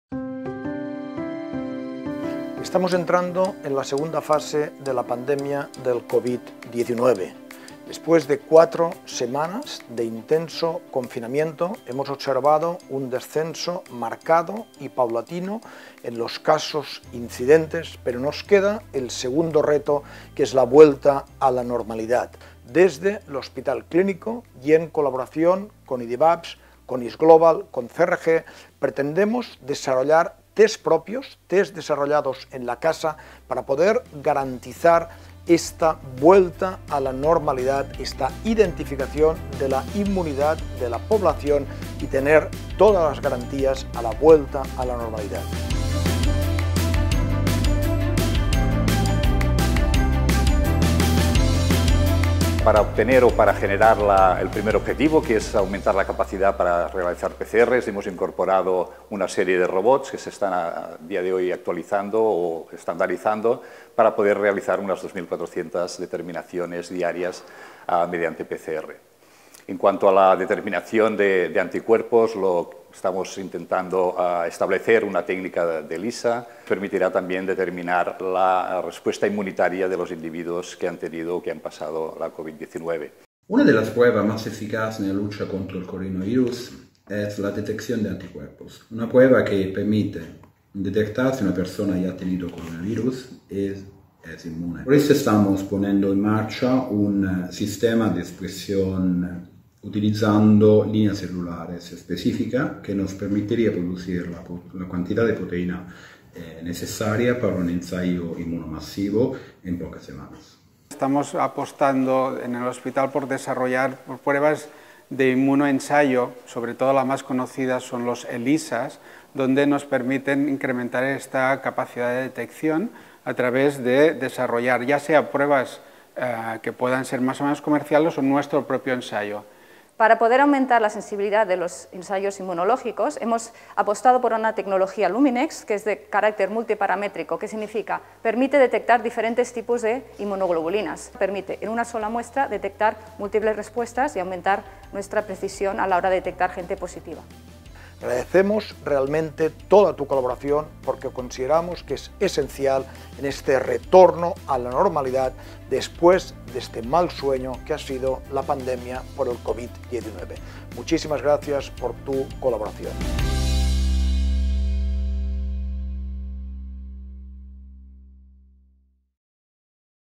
Proyecto Diagnóstico COVID-19
El proyecto "Diagnóstico COVID-19" tiene como objetivo aumentar la capacidad de realizar tests PCR para la COVID-19 y la puesta a punto técnicas de detección de anticuerpos frente al virus.

En el proyecto participan los laboratorios de microbiología (Dr. Jordi Vila) y de inmunología (Dr. Manel Juan) del Hospital Clínic, con la colaboración de los centros de investigación IDIBAPS (Dr. Pere Santamaria), ISGlobal (Dra. Carlota Dobaño) y CRG (Dr. Carlo Carolis).